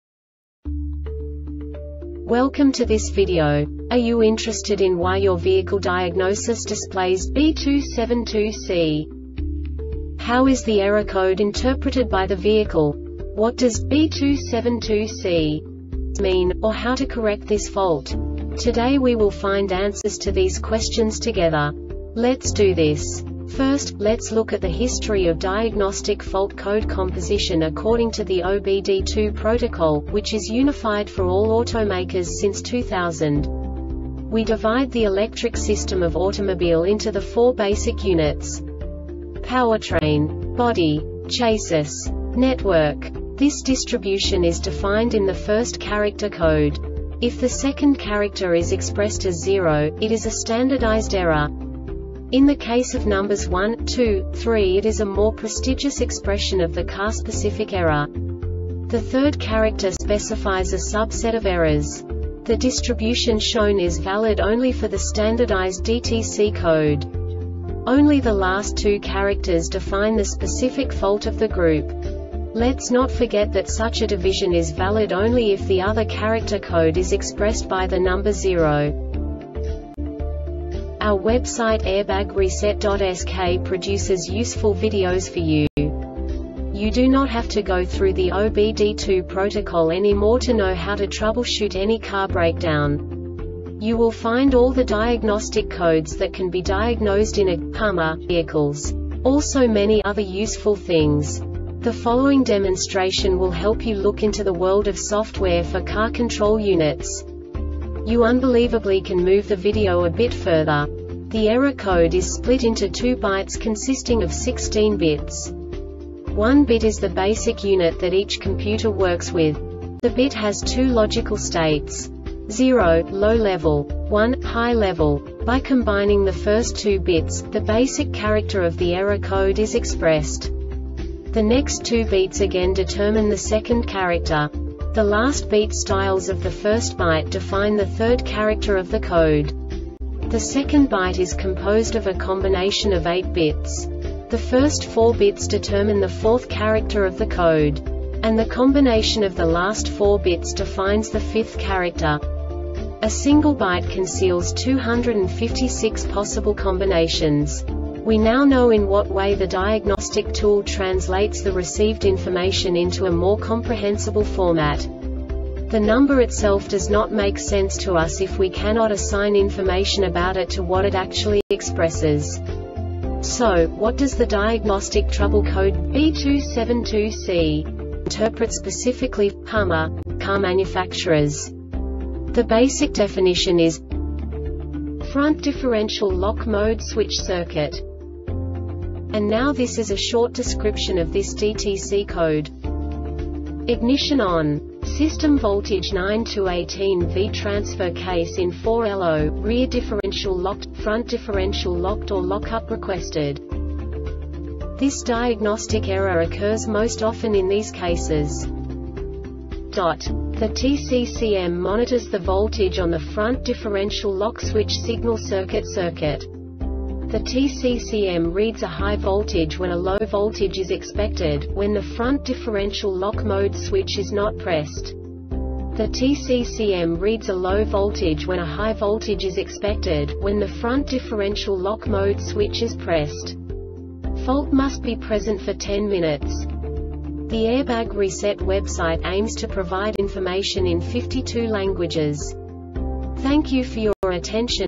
DTC Hummer B272C Short Explanation
Contents:
0:21 Basic DTC analysis according to OBD2 protocol standard.
1:48 Insight into programming
2:58 A diagnostically understandable description of the Diagnostic Trouble Code
3:05 Basic error definition

Make
HUMMER
Code
B272C
Definition
Front Differential Lock Mode Switch Circuit
Description
Ignition ON System voltage 9-18 V Transfer case in 4LO, rear differential locked, front differential locked or lockup requested
Cause
The TCCM monitors the voltage on the front differential lock switch signal circuit circuit
The TCCM reads a high voltage when a low voltage is expected, when the front differential lock mode switch is NOT pressed
The TCCM reads a low voltage when a high voltage is expected, when the front differential lock mode switch is pressed
Fault must be present for 10 minutes